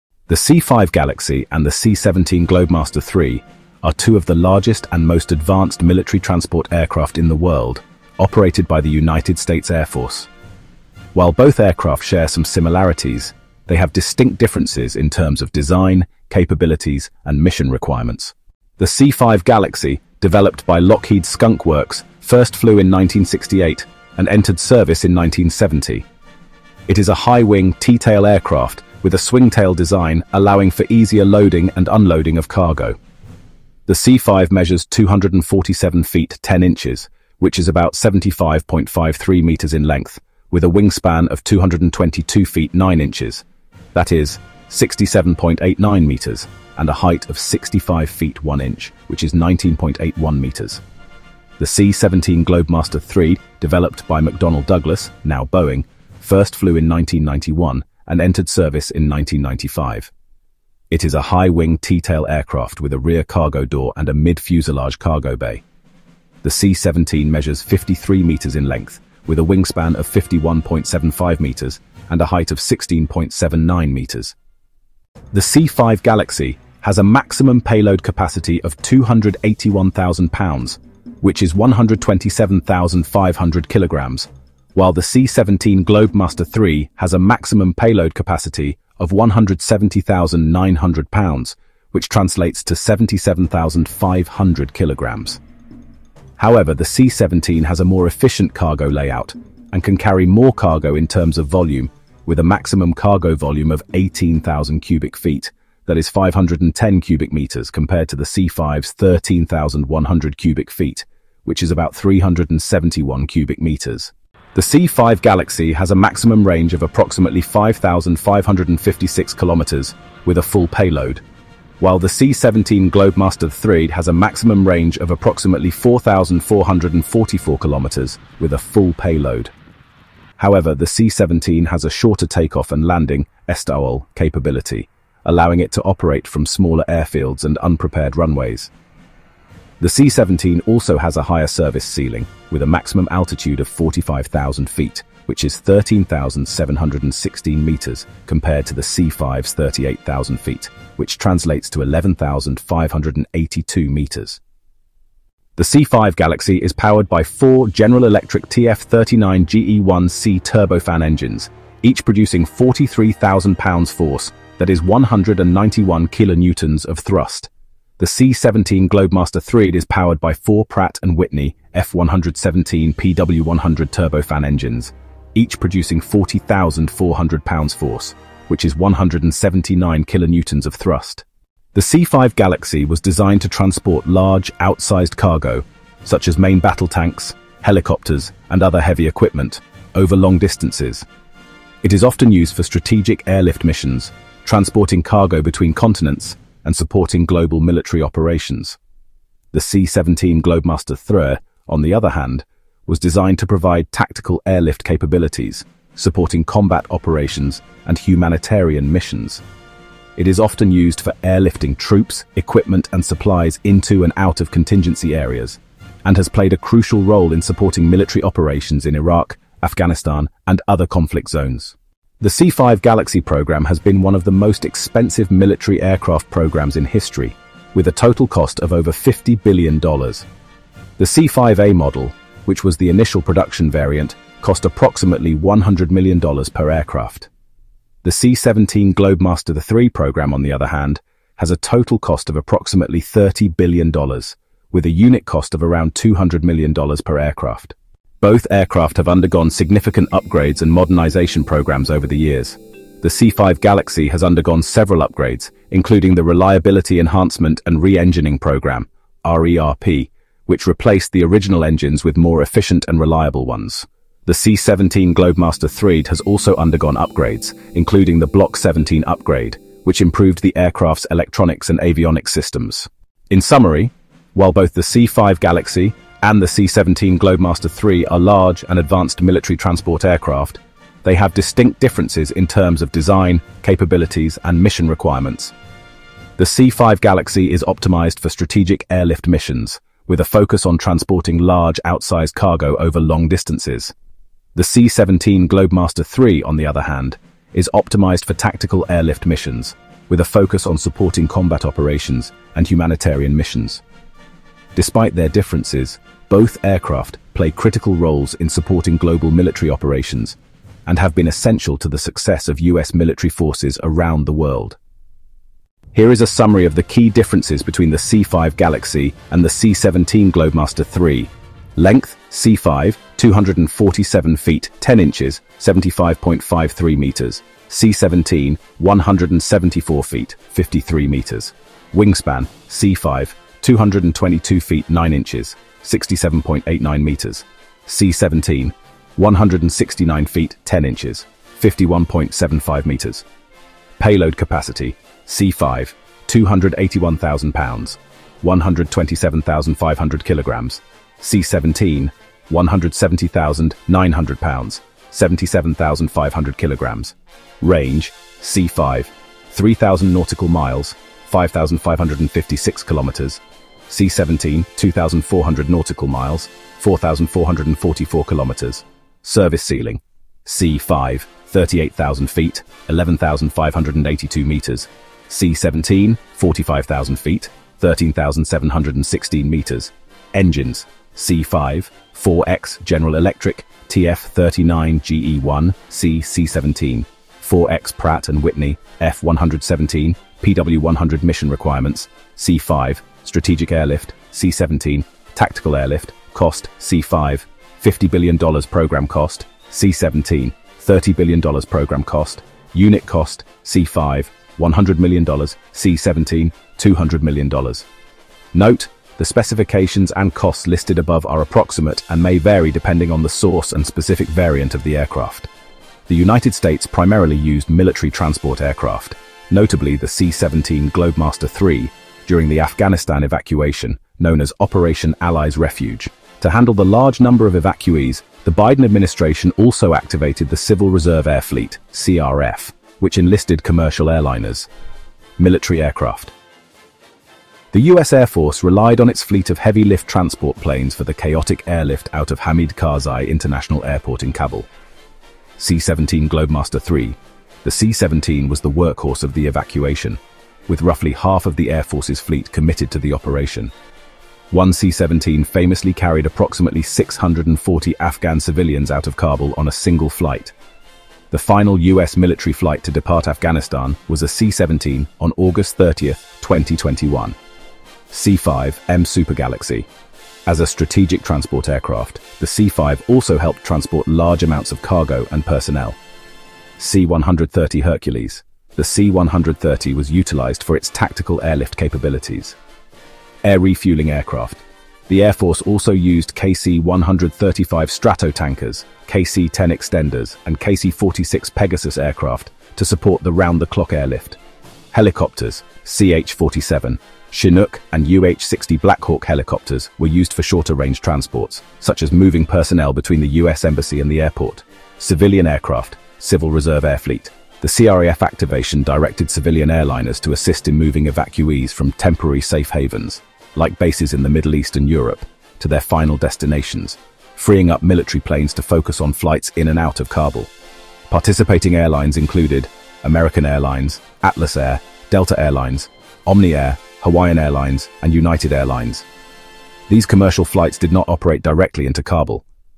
C-5 Galaxy vs C-17 Globemaster III: Key Differences Between these Two Giant Strategic Airlifters
The United States primarily used military transport aircraft, notably the

C-17 Globemaster III, during the Afghanistan evacuation, known as Operation Allies Refuge. To handle the large number of evacuees, the Biden administration also activated the Civil Reserve Air Fleet (CRAF), which enlisted commercial airliners.

Military aircraft

The U.S. Air Force relied on its fleet of heavy-lift transport planes for the chaotic airlift out of Hamid Karzai International Airport in Kabul.

C-17 Globemaster III: The C-17 was the workhorse of the evacuation, with roughly half of the Air Force's fleet committed to the operation. One C-17 famously carried approximately 640 Afghan civilians out of Kabul on a single flight. The final U.S. military flight to depart Afghanistan was a C-17 on August 30, 2021.

C-5M Super Galaxy: As a strategic transport aircraft, the C-5 also helped transport large amounts of cargo and personnel.

C-130 Hercules: The C-130 was utilized for its tactical airlift capabilities.

Air refueling aircraft: The Air Force also used KC-135 Stratotankers, KC-10 Extenders, and KC-46 Pegasus aircraft to support the round-the-clock airlift.

Helicopters: CH-47 Chinook and UH-60 Black Hawk helicopters were used for shorter-range transports, such as moving personnel between the U.S. embassy and the airport.

Civilian aircraft (Civil Reserve Air Fleet)

The CRAF activation directed civilian airliners to assist in moving evacuees from temporary safe havens, like bases in the Middle East and Europe, to their final destinations, freeing up military planes to focus on flights in and out of Kabul.

Participating airlines included: American Airlines, Atlas Air, Delta Air Lines, Omni Air, Hawaiian Airlines, and United Airlines.

These commercial flights did not operate directly into Kabul.

The C-5 Galaxy and C-17 Globemaster III are both large military transport aircraft, but they have distinct differences:

Design and Size:

C-5 Galaxy: Larger and heavier, with a length of 247 ft 1 in (75.3 m) and a maximum takeoff weight of 837,000 lb (379,600 kg).

C-17 Globemaster III Smaller, with a length of 174 ft 1 in (53 m) and a maximum takeoff weight of 585,000 lb (265,350 kg).

Range and Speed:

C-5 Galaxy Has a longer range of approximately 4,800 nmi (8,900 km) and a higher cruise speed of around Mach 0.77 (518 mph or 834 km/h).

C-17 Globemaster III: Has a range of approximately 2,400 nmi (4,400 km) and a cruise speed of around Mach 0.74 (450 mph or 724 km/h).

Cargo Capacity:

C-5 Galaxy: Can carry more cargo, with a maximum payload of 281,000 lb (127,500 kg).
C-17 Globemaster III: Has a maximum payload of 170,900 lb (77,520 kg).

Tactical Capabilities:
C-5 Galaxy: Primarily designed for strategic airlift, with limited tactical capabilities.

C-17 Globemaster III :Designed for both strategic and tactical airlift, with the ability to operate from short, unprepared runways.

Service Life and Maintenance:

C-5 Galaxy: Has undergone several upgrades and modernization programs to extend its service life.

C-17 Globemaster III: Designed with maintainability in mind, featuring a robust design and easy maintenance access.

Production and Operators:

C-5 Galaxy: Produced from 1968 to 1973, with 131 aircraft built. Operated by the US Air Force.

C-17 Globemaster III

Produced from 1991 to 2015, with 279 aircraft built. Operated by several countries, including the US, UK, Canada, and Australia.

In summary, the C-5 Galaxy is a larger, heavier aircraft with a longer range and higher speed, while the C-17 Globemaster III is smaller, more maneuverable, and designed for both strategic and tactical airlift.